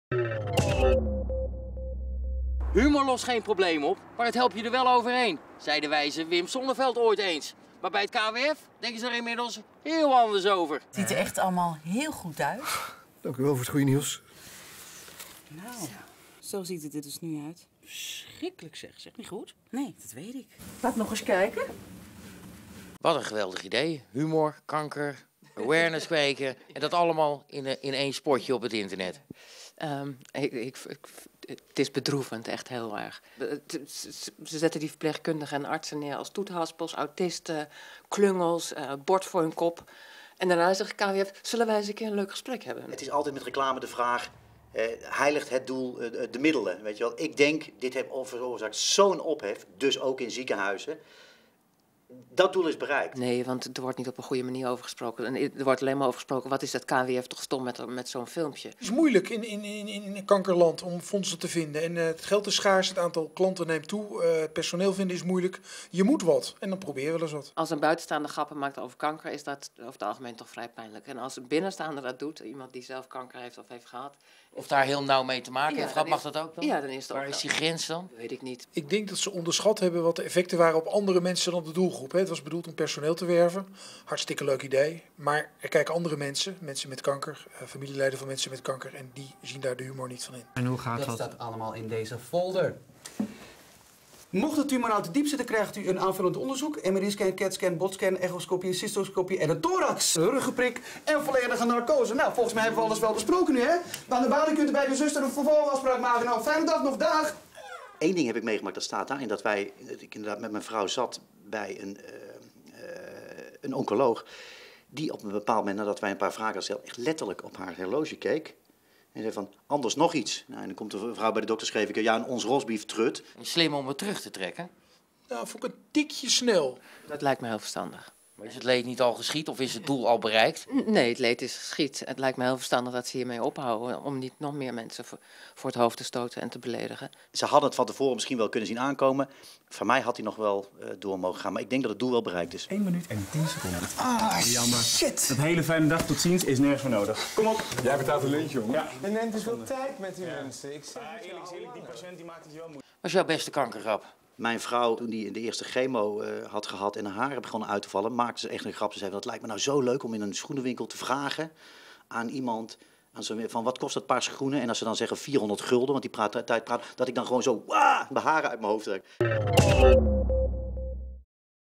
Kankerfilmpje van KWF: is dit grappig?
PowNews-verslaggever Tom Staal vroeg onder meer Kluun wat hij vindt van de inmiddels teruggetrokken controversiele campagne van KWF Kankerbestrijding.